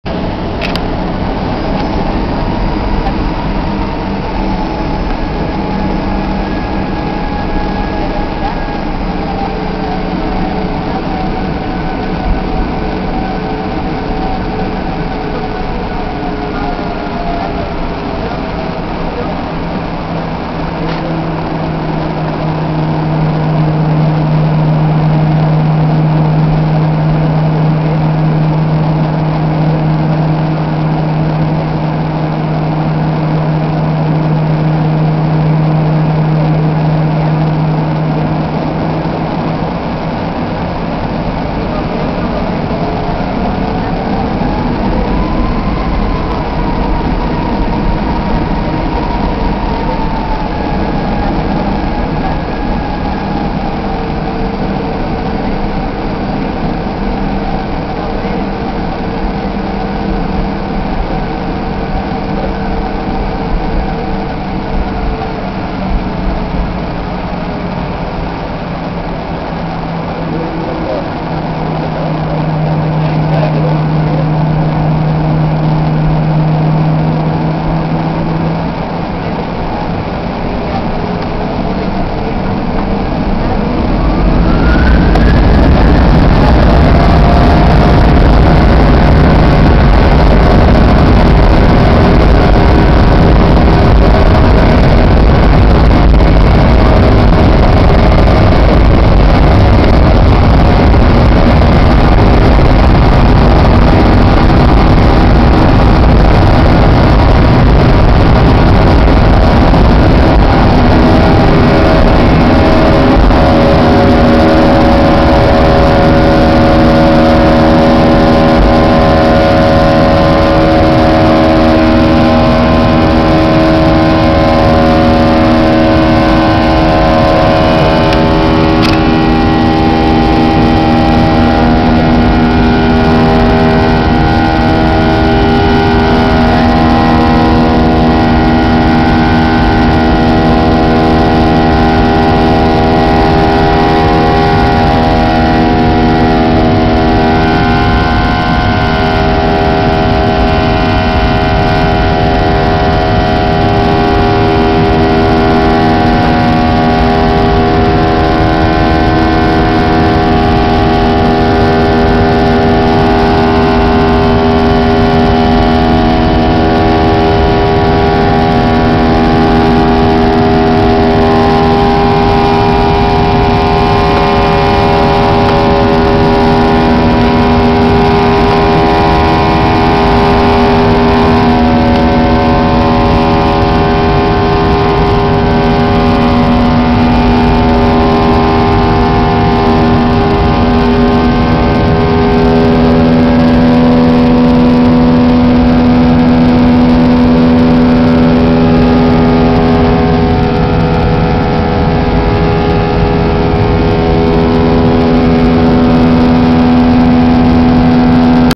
Taking off from Tegucigalpa to Atlanta on Nov. 21 2009
Taking off from Toncontin to Hartsfield-Jackson Airport in ATL